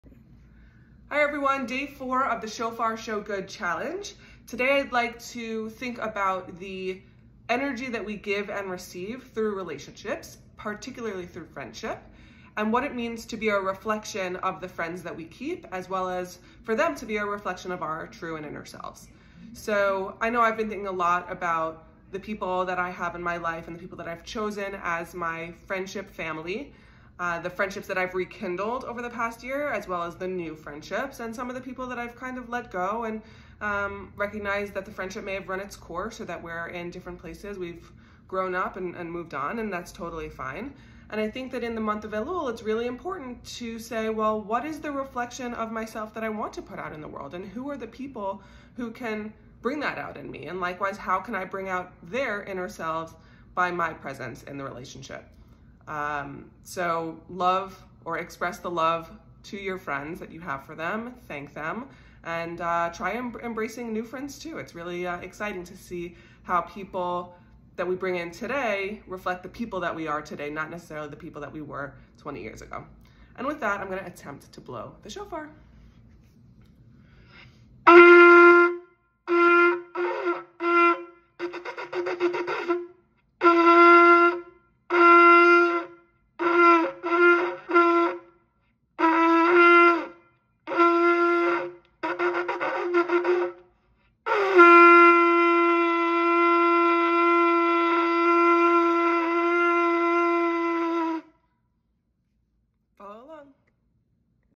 ShofarShoGood Challenge 5783 - Day 4
Day 4 of the Shofar ShoGood Challenge. Today I’m thinking about the energy we give and receive through relationships, particularly through friendship. It’s a great exercise to define what friendship means to you, what kinds of friends you need and desire to have in your life, and what kind of friend do you want to be in the year to come. What do you think?